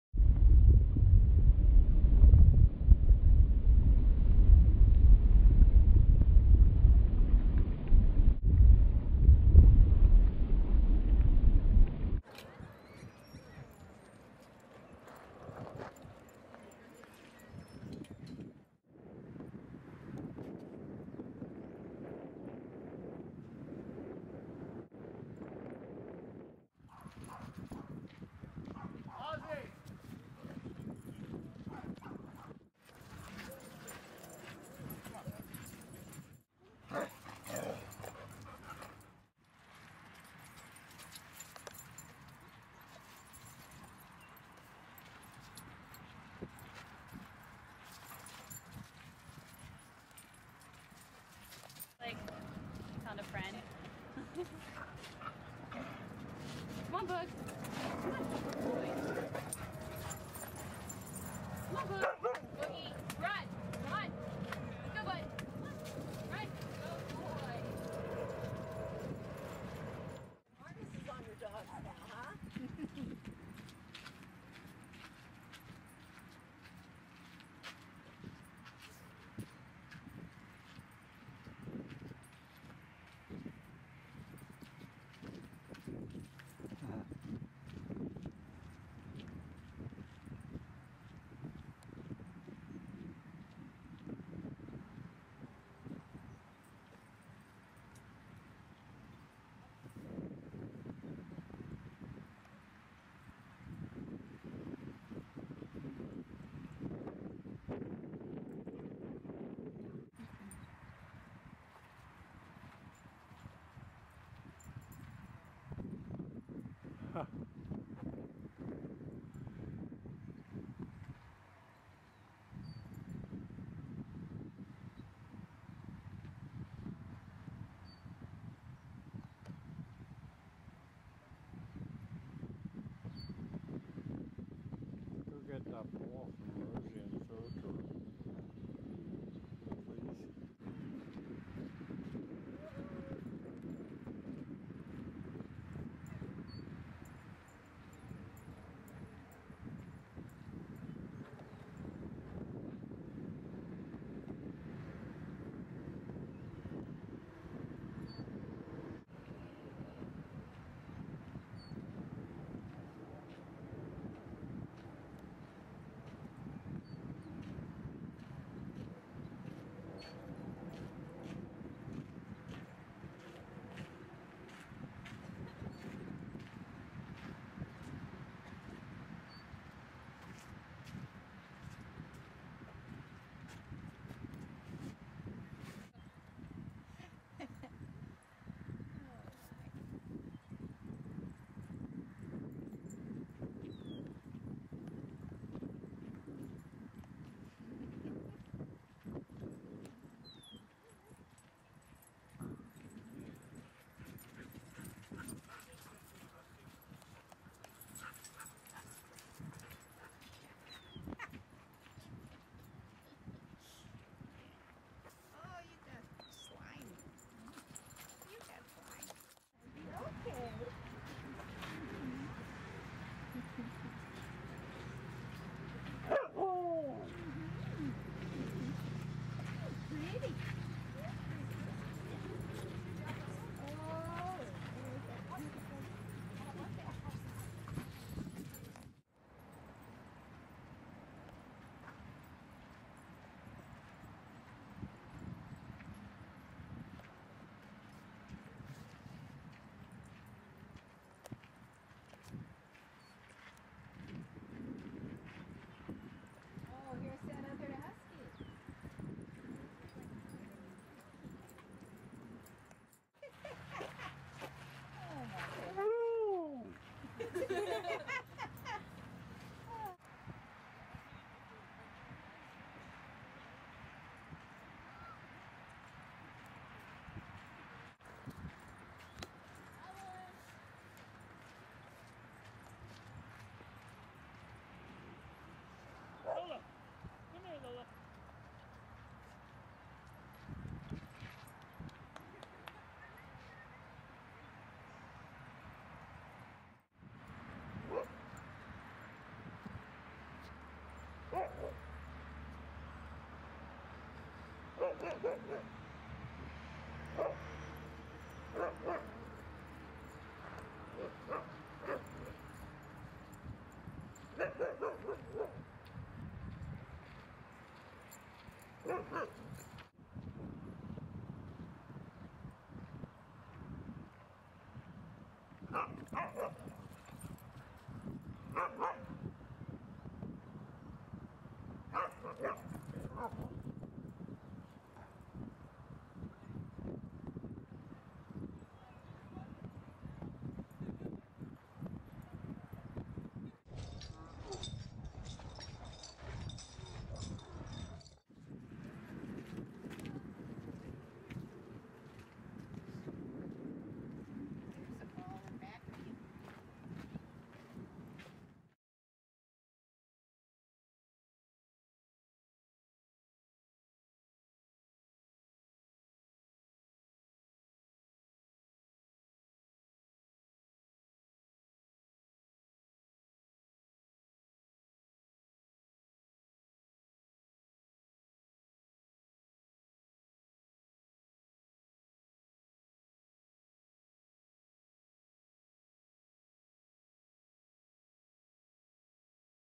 Rosy And Rocky Survive A Cloudy Day At Seal Point DP
Rosy and Rocky have fun in Seal Point Dog Park. Rosy and Rocky go to the dock. Rosy and Rocky play together briefly before leaving.